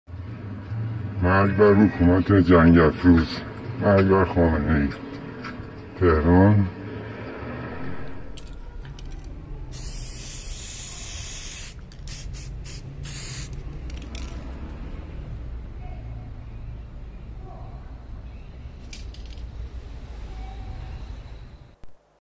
شعارنویسی شهروندان علیه جنگ‌افروزی جمهوری اسلامی و علی خامنه‌ای
برشی از برنامه پخش زنده در تلویزیون کانال جدید
چهارشنبه ۲۵ مهر ۱۴۰۳ - ۱۶ اکتبر ۲۰۲۴